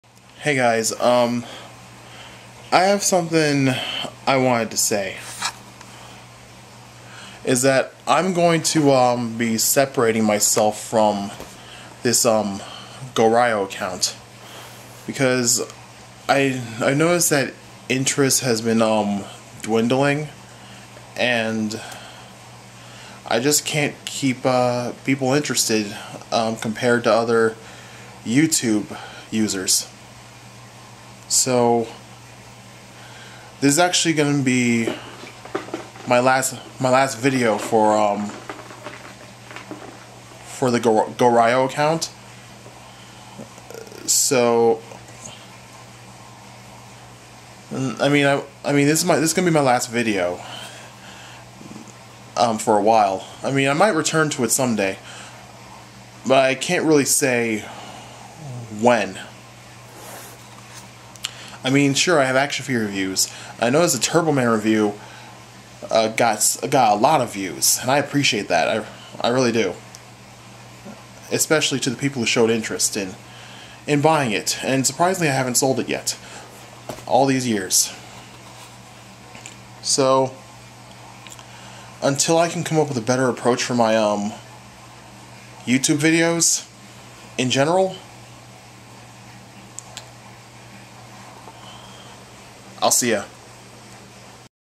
Abandoning This Account
For the record, I'm not deleting anything. I'm just abandoning this account until I can come up with a better approach, i.e., better video editing software and some way to keep people interested.